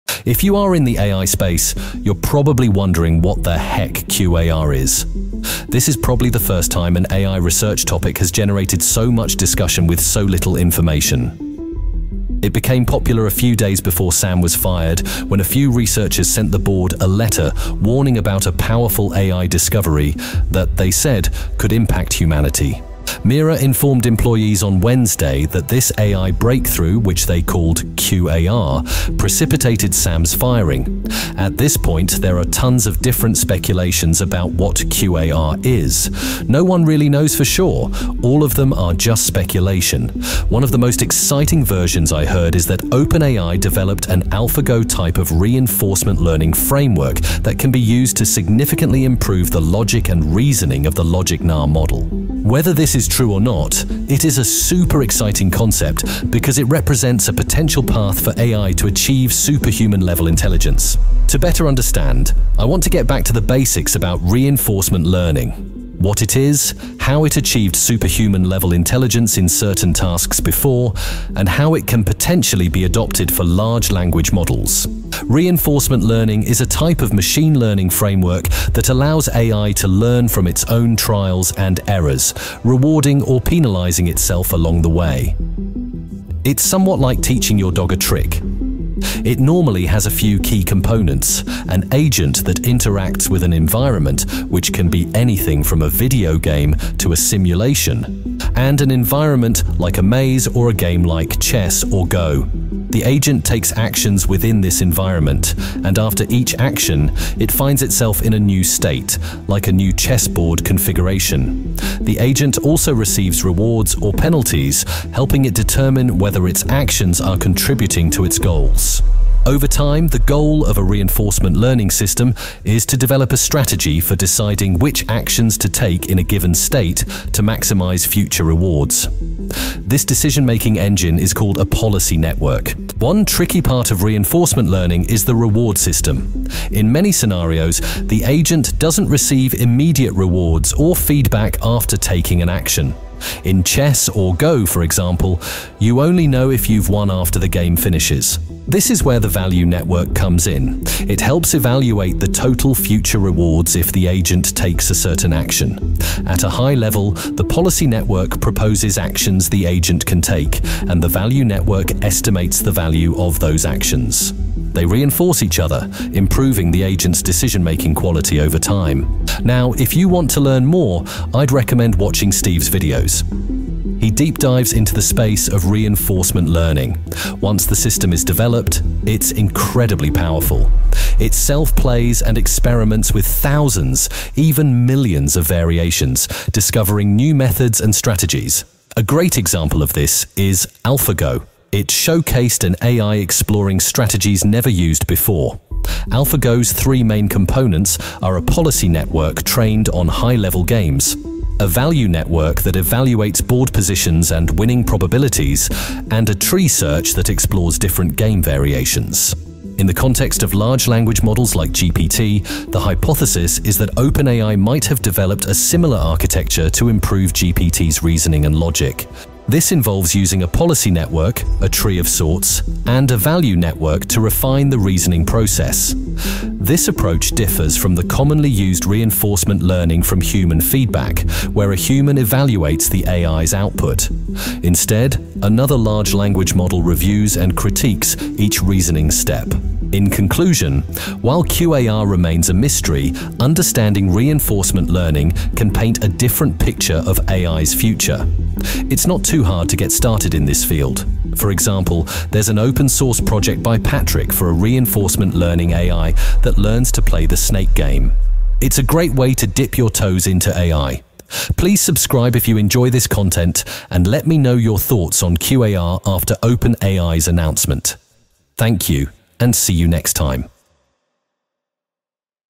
AGI has been ACHIEVED Q* Announced by OpenAI
"In this video, we delve into the groundbreaking news from the tech world: OpenAI and ChatGPT's announcement of achieving a monumental milestone in artificial intelligence - the realization of Artificial General Intelligence (AGI). We explore what this achievement means for the future of technology and how it could transform our interaction with the digital world. We also discuss the challenges and opportunities brought by this new development, taking a look at its potential impact across various industries and society as a whole. Join us as we navigate the exciting world of AGI and how it may shape our future."